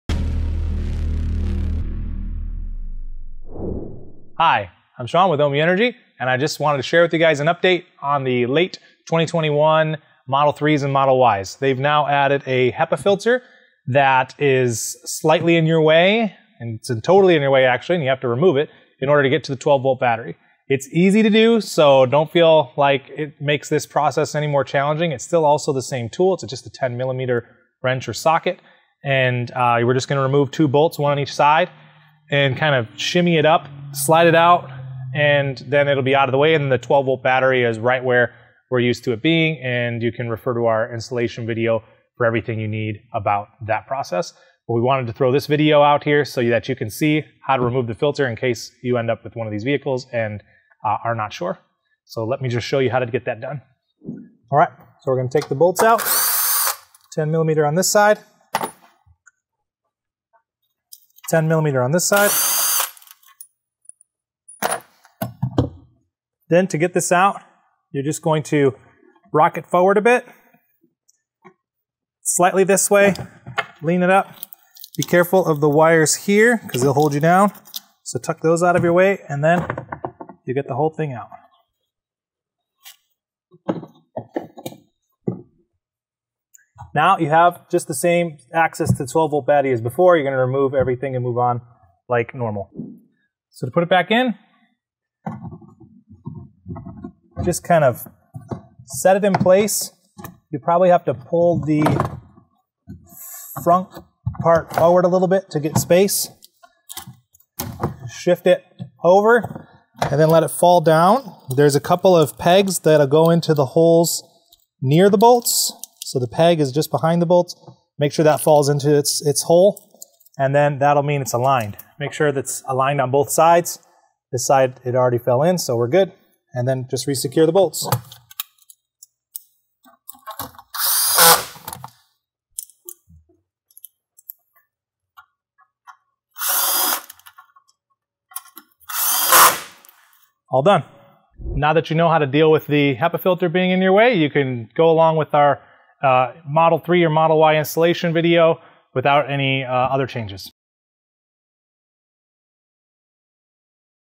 Installation Update for Late 2021–March 2022 Tesla Model 3/Y - 12V Battery Installation (Ohmmu)
Here's a quick update on the install process to access the 12V battery on Tesla Model 3/Y vehicles with the new HEPA filter (late 2021–March 2022 models). 12V battery install videos below ▼

▬ Additional Support  ▬▬▬▬▬▬▬▬▬▬